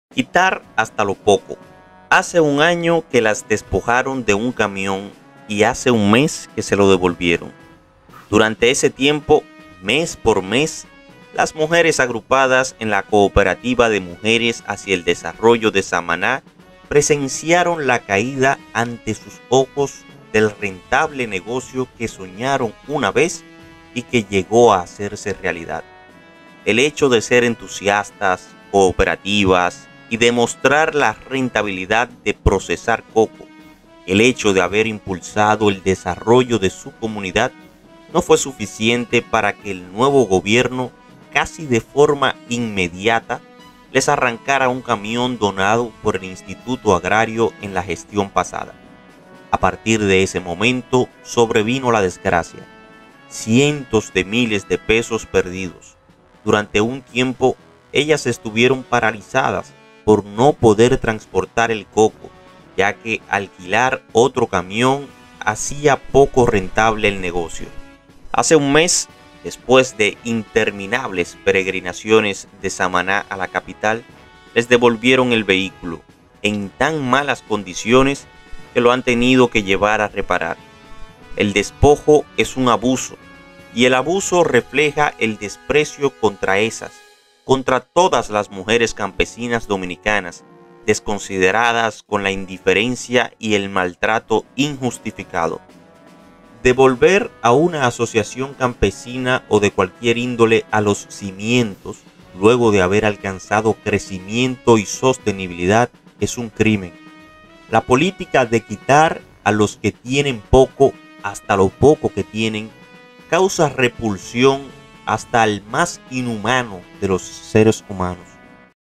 |Quiero que sepas| Quitar hasta lo poco
Hace un año que las despojaron de un camión y hace un mes que se lo devolvieron.

Durante ese tiempo, mes por mes, las mujeres agrupadas en la Cooperativa de Mujeres Hacia el Desarrollo de Samaná presenciaron la caída ante sus ojos del rentable negocio que soñaron una vez y que llegó a hacerse realidad.

Puedes encontrar a Ojalá en:
•Ojalá en Twitter:

•Ojalá Comunidad:
Alegría de ser diferente
Únete. Haz un donativo.